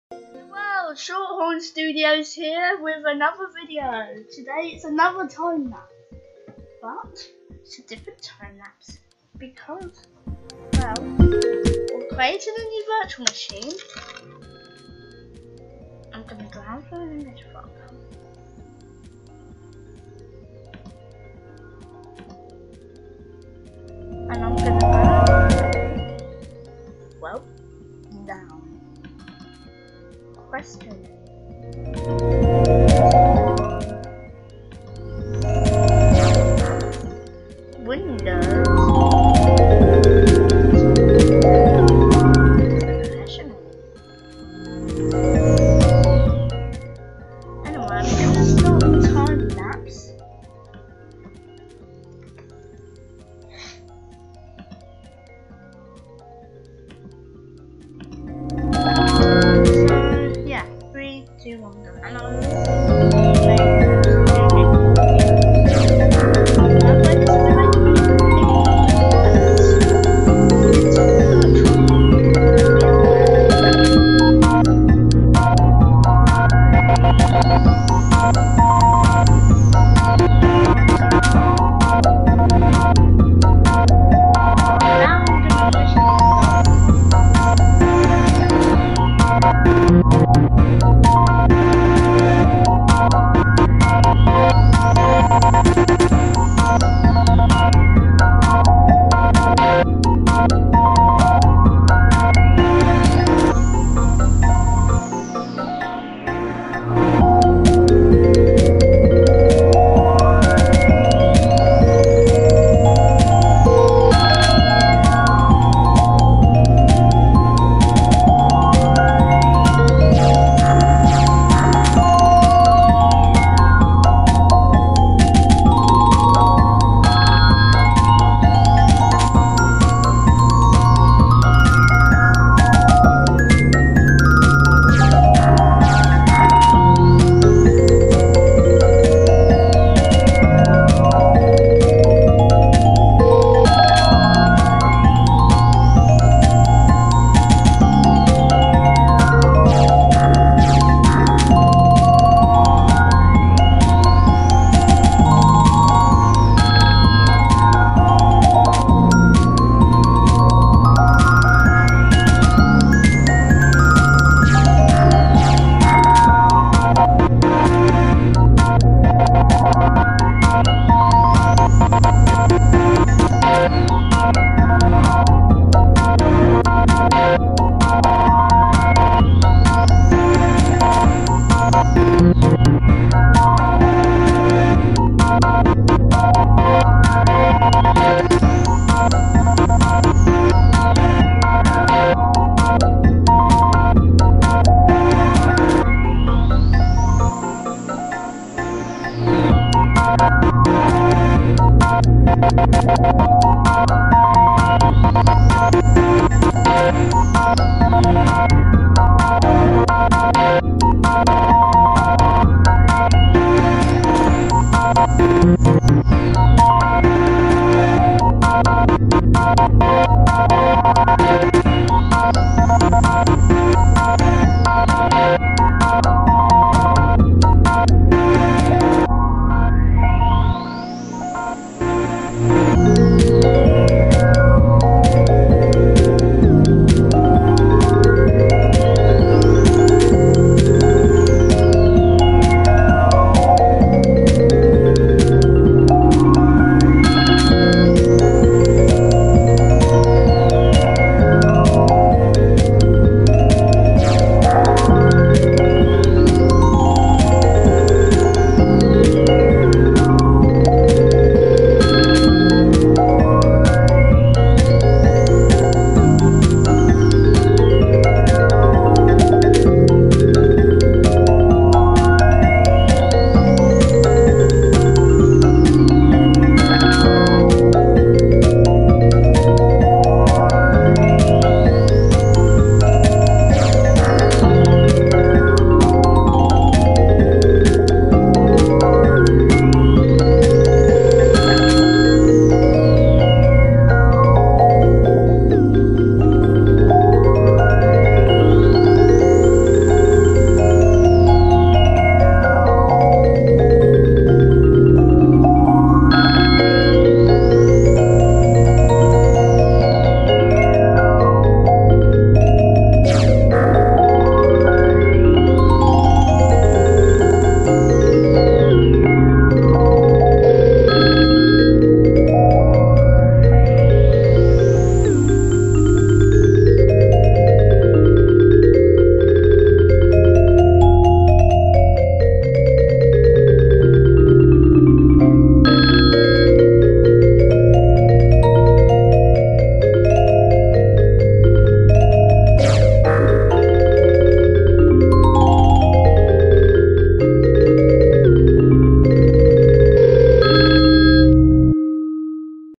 Windows 2000 Installation [EPILEPSY WARNING NEAR END]
Windows 2000, the legendary and 1st Windows NT version to be in the golden era. This is the last version to be paired with a 9x version (Windows ME).